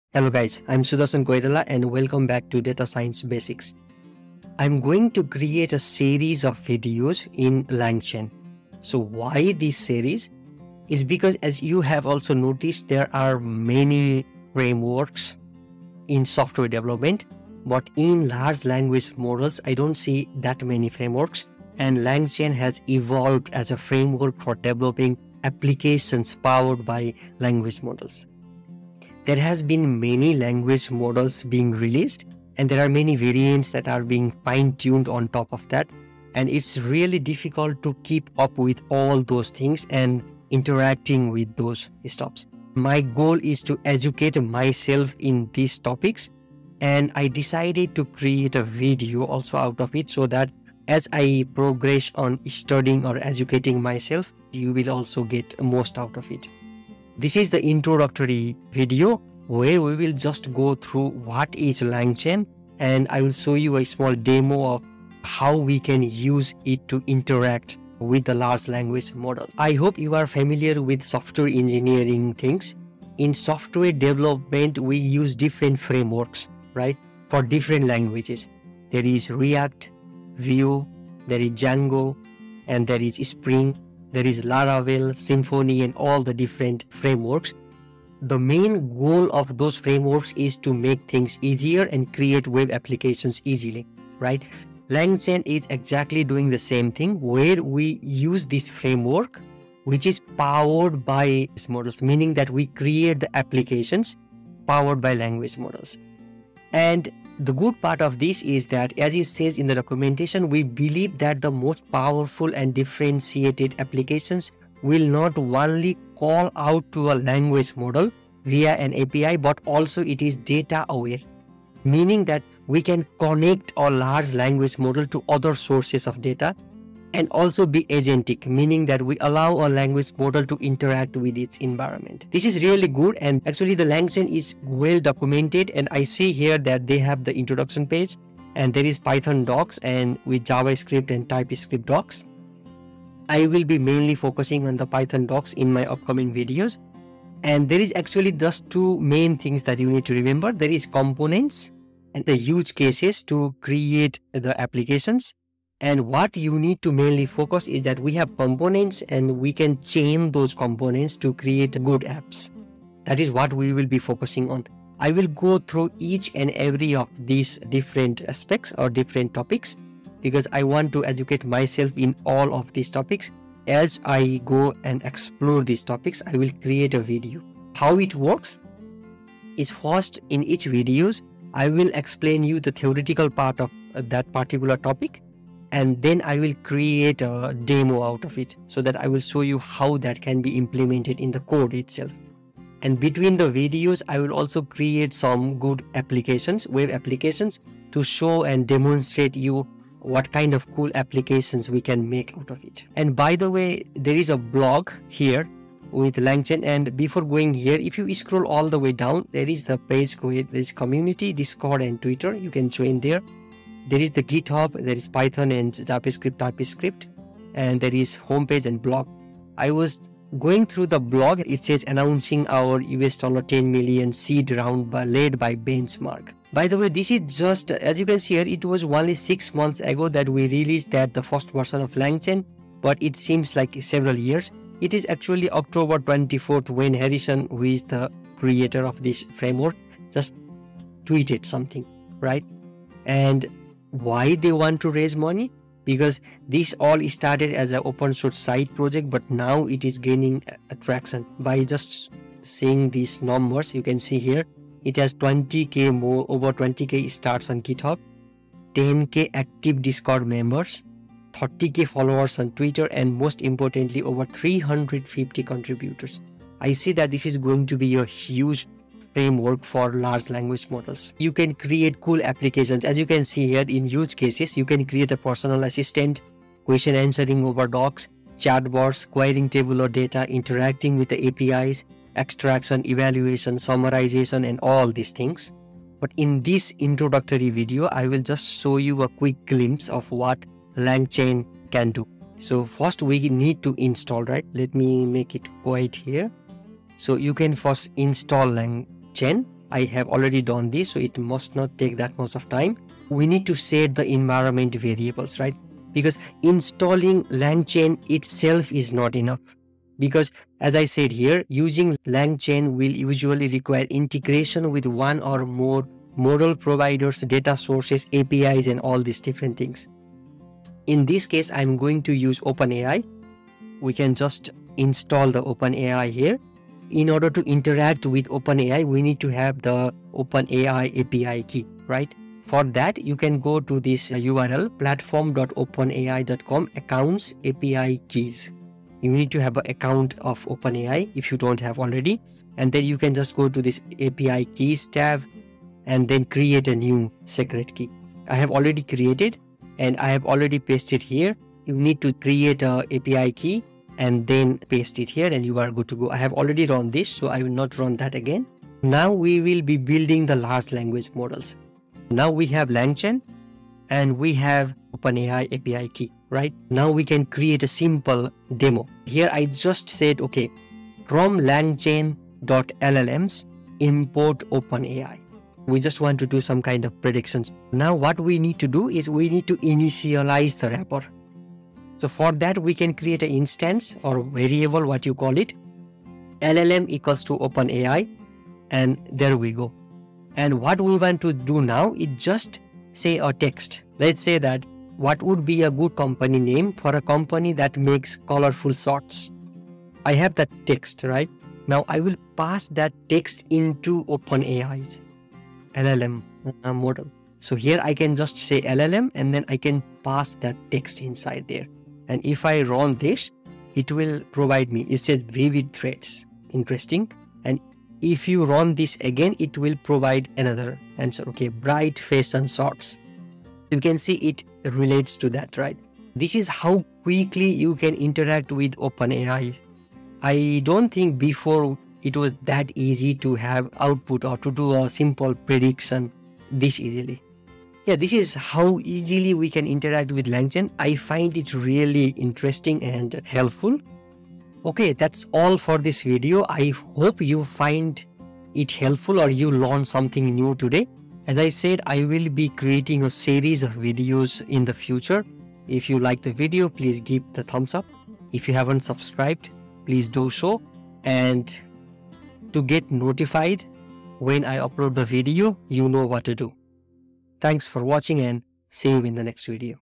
What is LangChain ? | LangChain Intro #1
LangChain is a framework for developing applications powered by language models. This is the first video in the series of video to follow about LangChain and building cool applications out of it.